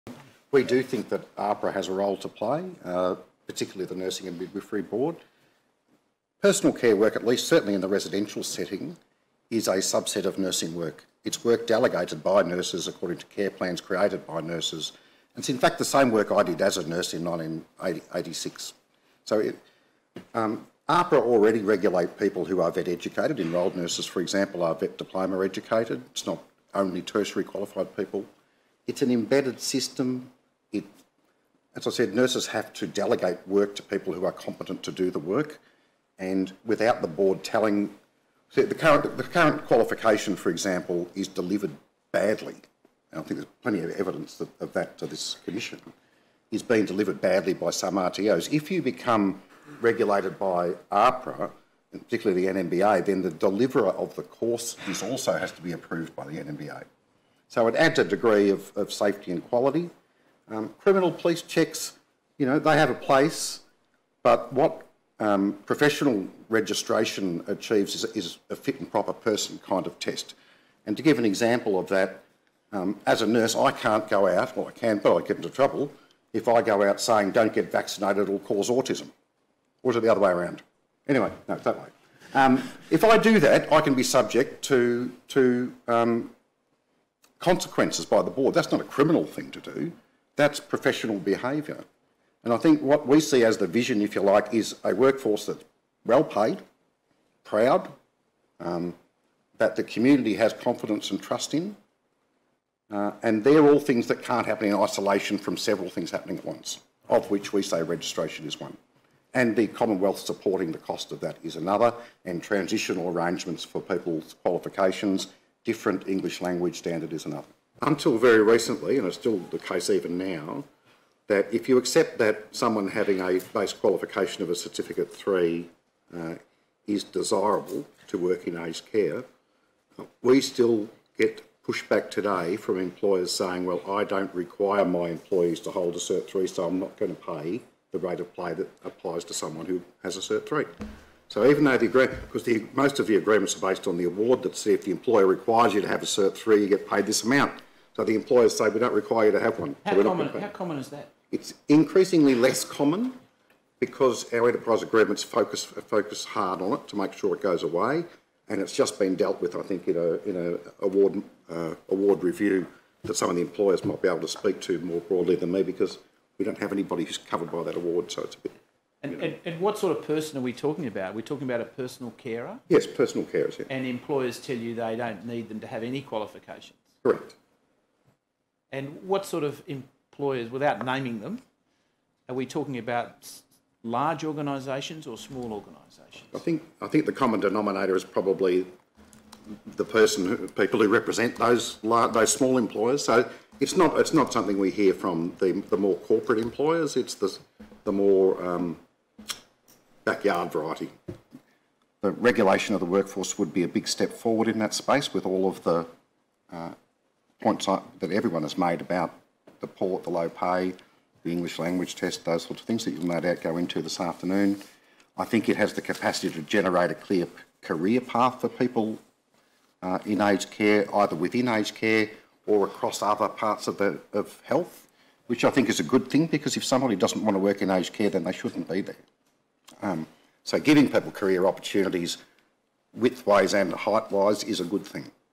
Assistant Secretary Paul Gilbert on carers [Aged care royal commission]
Assistant Secretary Paul Gilbert talks about AHPRA registration and education for personal care workers at a Melbourne hearing of the Royal Commission into Aged Care Quality and Safety on Wednesday 16 October 2019.

TRANSCRIPT:

We do think that AHPRA has a role to play, particularly the Nursing and Midwifery Board.  Personal care work, at least, certainly in the residential setting, is a subset of nursing work.  It’s work delegated by nurses according to care plans created by nurses.  It’s, in fact, the same work I did as a nurse in 1986.

So AHPRA already regulate people who are VET-educated;  enrolled nurses, for example, are VET diploma educated.  It’s not only tertiary qualified people.  It’s an embedded system.  As I said, nurses have to delegate work to people who are competent to do the work and without the board telling – the current qualification, for example, is delivered badly.  I think there’s plenty of evidence of that to this Commission, is being delivered badly by some RTOs.

If you become regulated by AHPRA, and particularly the NMBA, then the deliverer of the course also has to be approved by the NMBA.  So it adds a degree of safety and quality.  Criminal police checks, you know, they have a place but what professional registration achieves is a fit and proper person kind of test.  To give an  example of that, as a nurse, I can’t go out – or I can but I would get into trouble, if I go out saying don’t get vaccinated, it will cause autism.  Or is it the other way around.  Anyway, not it’s that way.  If I do that, I can be subject to – to consequences by the board.  That’s not a criminal thing to do.  That’s professional behaviour.

And I think what we see as the vision, if you like, is a workforce that’s well paid, proud, that the community has confidence and trust in and they’re all things that can’t happen in isolation from several things happening at once, of which we say registration is one, and the Commonwealth supporting the cost of that is another and transitional arrangements for people’s qualifications, different English language standard is another.

Until very recently – and it’s still the case even now – that if you accept that someone having a base qualification of a certificate 3 is desirable to work in aged care – we still get pushback today from employers saying, “Well, I don’t require my employees to hold a cert 3;  so I’m not going to pay the rate of pay that applies to someone who has a cert 3”.  So even though the – because most of the agreements are based on the award that say, if the employer requires you to have a cert 3, you get paid this amount.  So the employers say “We don’t require you to have one.  So we’re not going to pay”.

MR BOLSTER:   How common is that?

MR GILBERT:   It’s increasing less common, because our enterprise agreements focus hard on it, to make sure it goes away, and it’s just been dealt with, I think, in an award review that some of the employers might be able to speak to more broadly than me, because we don’t have anybody covered by that award.

MR BOLSTER:   And what sort of person are we talking about?  Are we talking about a personal carer?

MR GILBERT:   Yes.  Personal carers.  Yes.

MR BOLSTER:   And employers tell you they don’t need them to have any qualifications.

MR GILBERT:   Correct.

MR BOLSTER:   And what sort of employers – without naming them – are we talking about large organisations or small organisations?

MR GILBERT:   I think the common denominator is, probably, the person – people who represent those smaller employers.  So it’s not something we hear from the more-corporate employers;  it’s the more-backyard variety.

I think, as I said, the regulation of the workforce would be a big step forward in that space with all of the points that everyone has made about the poor – the low pay, the English language test, those sort of things that you’ll no doubt go into this afternoon.

I think it has the capacity to generate a clear career path for people in aged care, either within aged care or across other parts of health, which I think is a good thing because if somebody doesn’t want to work in aged care then they shouldn’t be there.  So giving people career opportunities width-ways and height-wise is a good thing.

CREDIT
Footage and transcript from the Royal Commission into Aged Care Quality and Safety